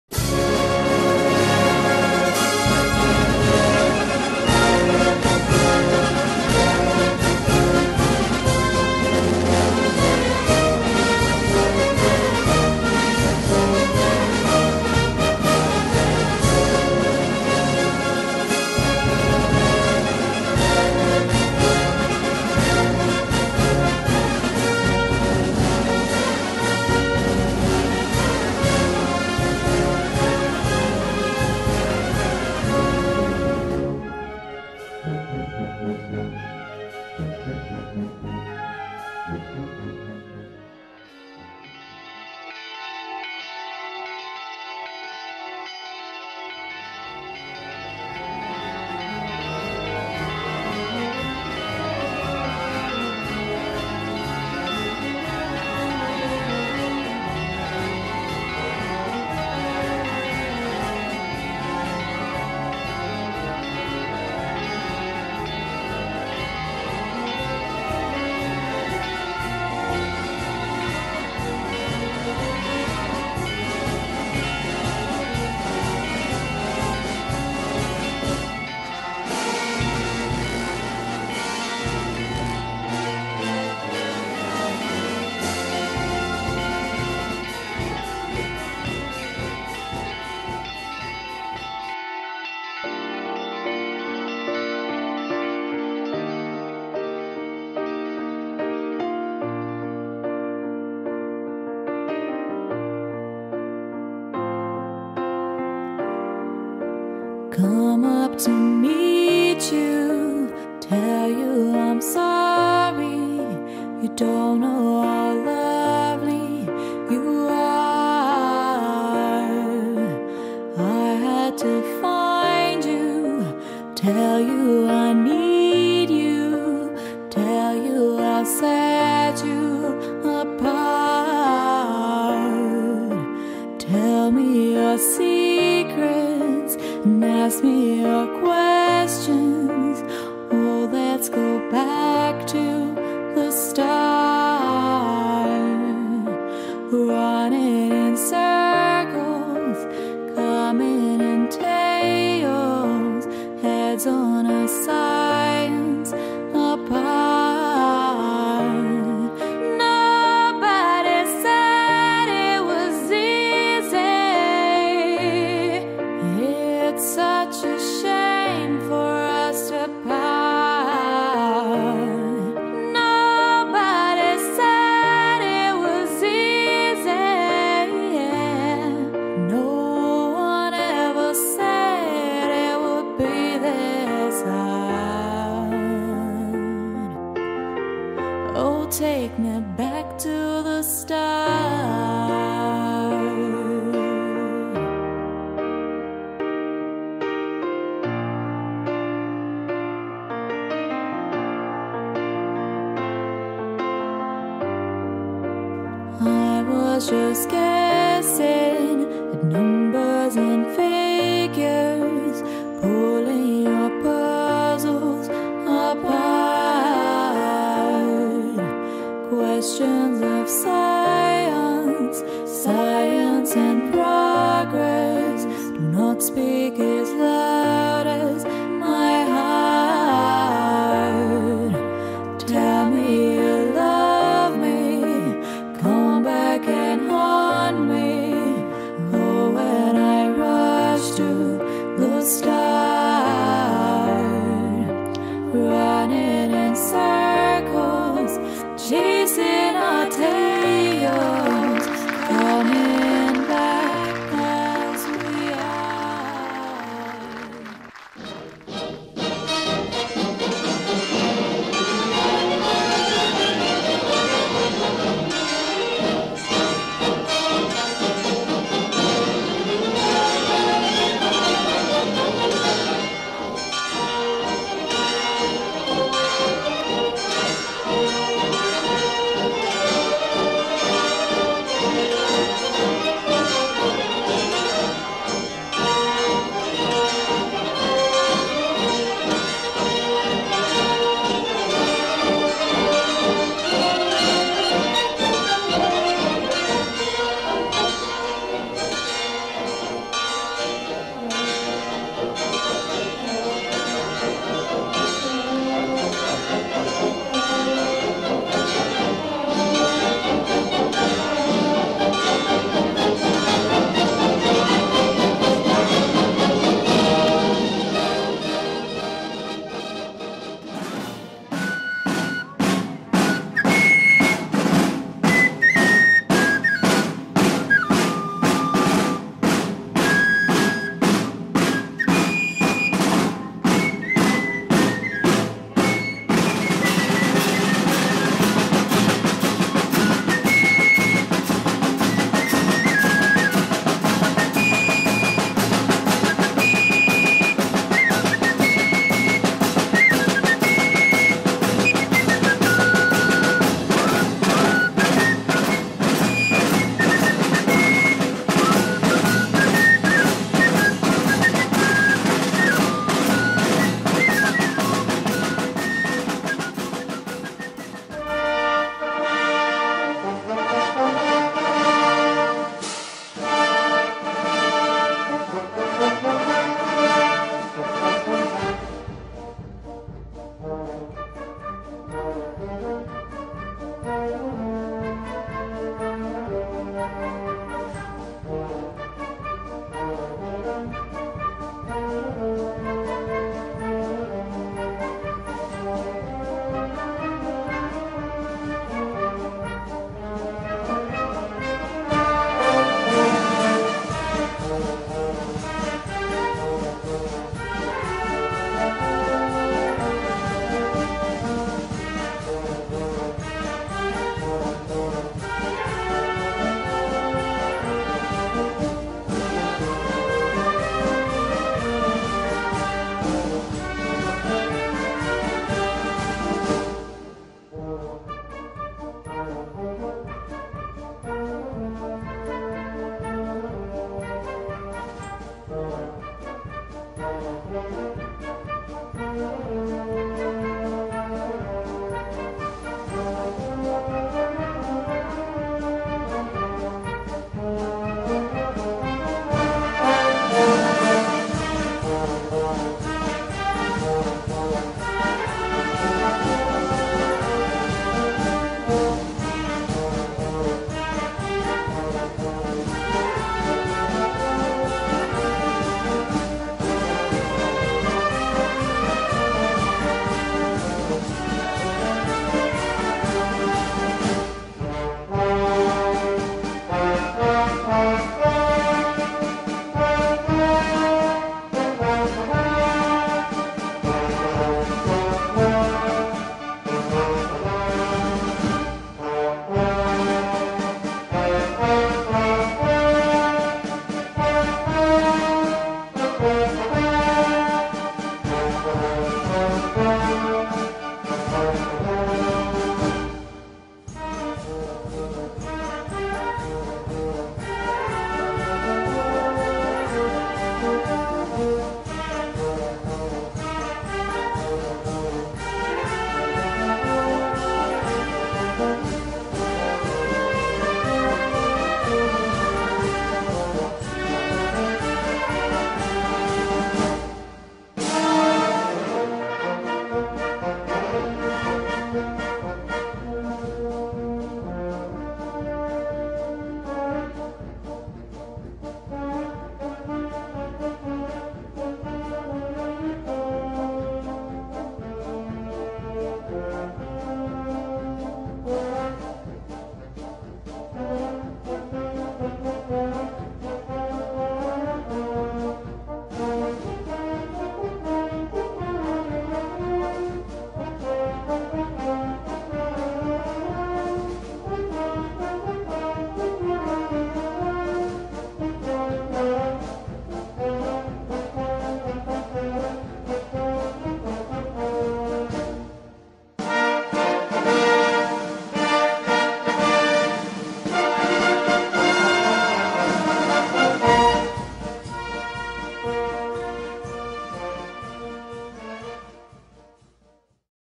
SANTI MEDICI 2019 LATERZA
LATERZA (TA), DOMENICA 29 SETTEMBRE 2019, FESTEGGIAMENTI
IN ONORE DEI SANTI MEDICI COSMA E DAMIANO.